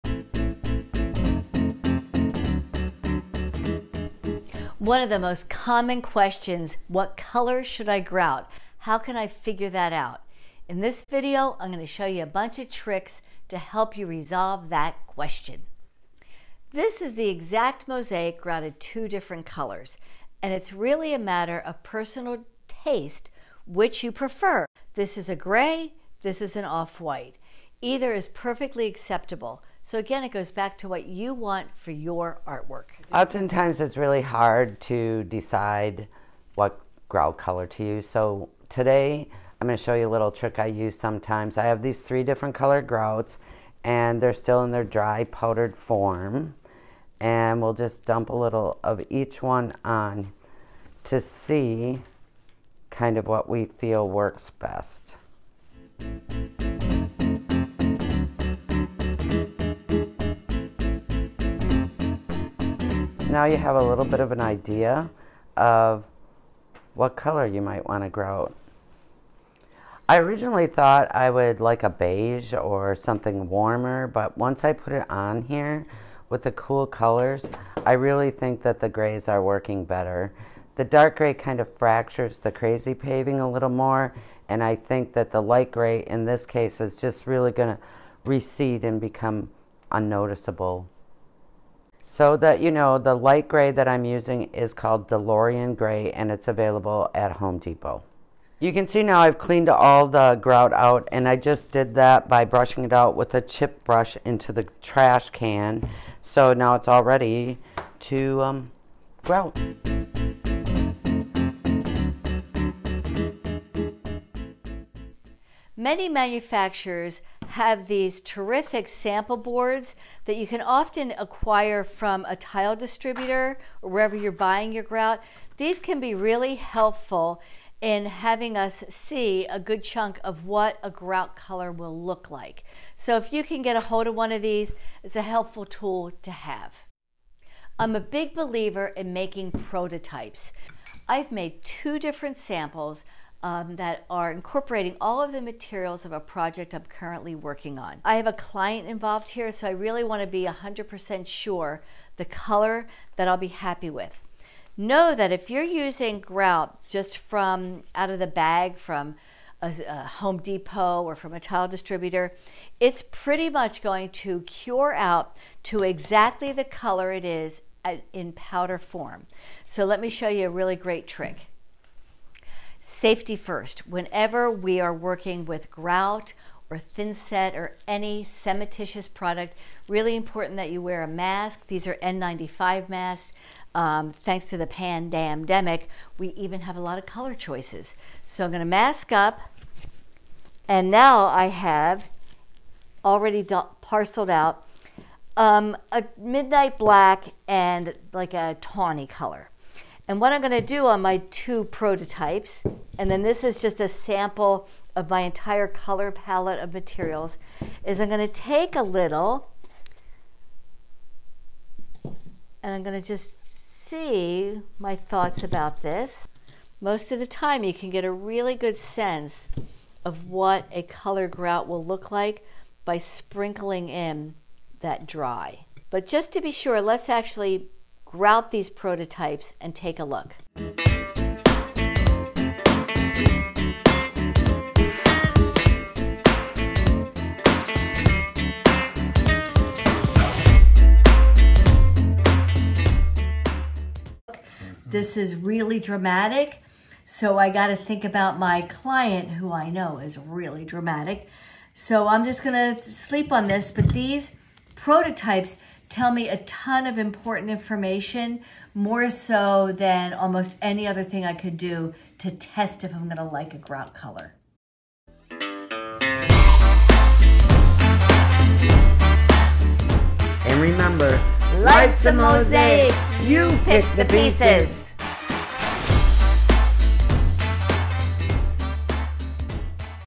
Selecting a Grout Color for your Mosaic Art.
The application of the grout that fills the gaps between the tesserae is the final act of creating the mosaic. The grout can transform the separate pieces of tesserae into a unified, coherent design.
Grouting can be the scariest and most rewarding part of a project. As you stare at hours of work a million things spin through your mind. How do I figure out what color is the most asked question!

In this video Bonnie and Kim share some tricks to help you with making the best selection for your project. You can purchase grout from home improvement stores and tile distributors. We prefer polymer fortified sanded grout. There are many good brands on the market.

Grout is a cement-based mixture.  The basic ingredients in grout are Portland cement, sand, and water, once mixed together a chemical hardening process begins. Grout has little adhesive properties and cannot be used to "stick" tiles in place.

You can purchase grout from home stores, many hardware stores, and of course amazon.  This link is sanded grout and similar color and small amount

Safety First! N95 Face Mask

Great Books: 300+ Mosaic
Guide to Mosaic techniques

other links